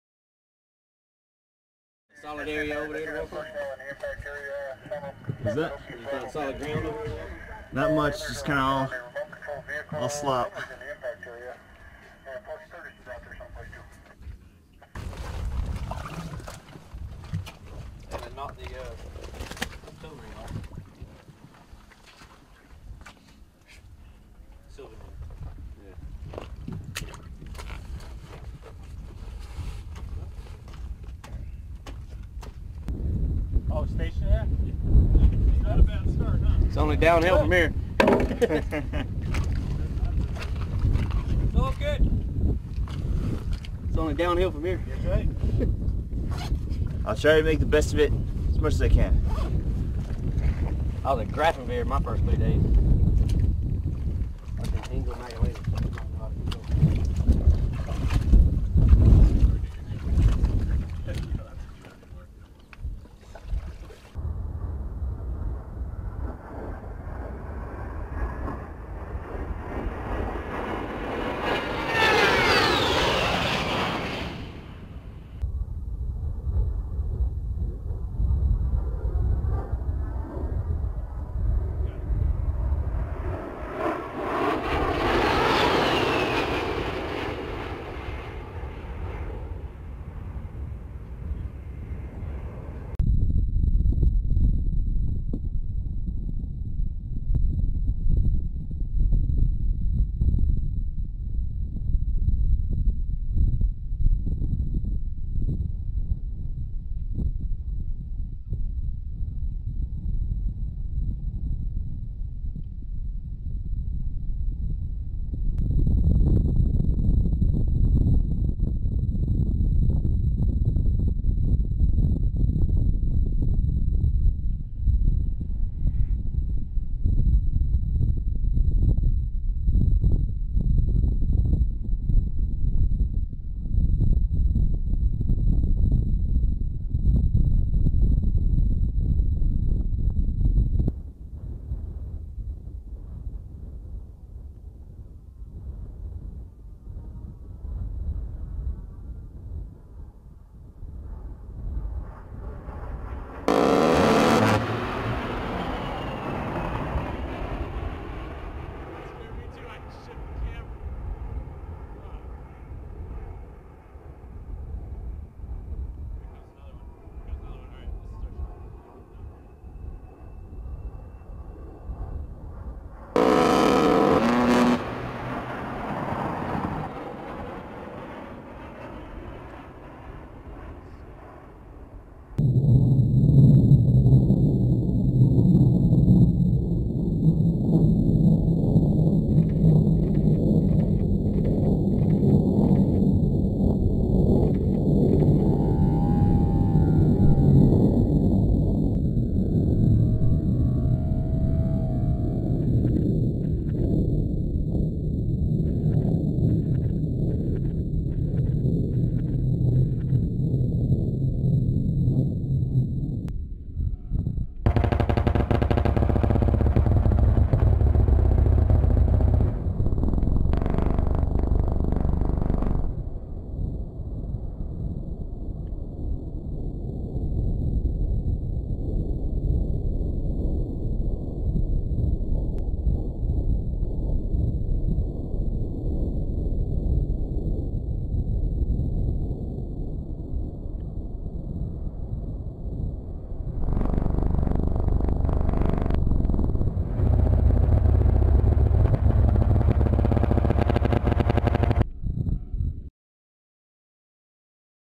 A-10 Warthogs Target Practice On Humvees • Slow Mo Hits
A-10 Warthogs from the 124th Fighter Wing practice targeting humvees at the Saylor Creek Training Range in Idaho on February 10, 2017.
Film Credits: SrA Connor Marth